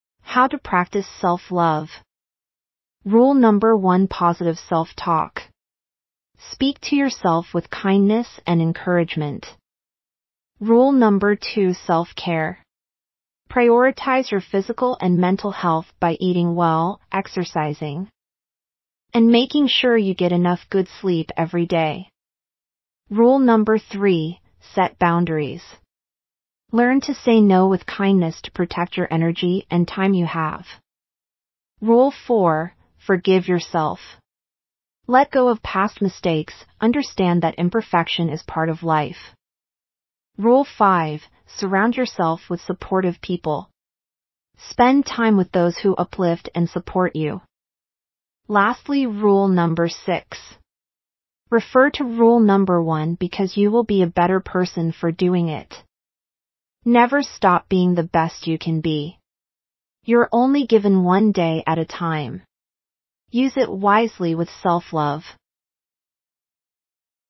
How to learn self Love.
6 rules to follow and help you learn how to have self-love.
Be the best you can be.